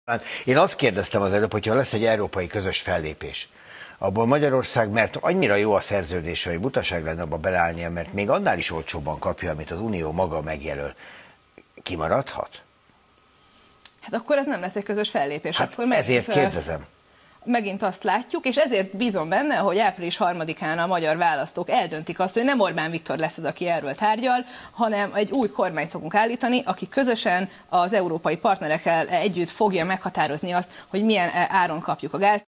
Cseh Katka nagyon gáz!
Olcsó gáz nem kell, csak olyan amit drágán vesztek és adtok el a fogyasztóknak!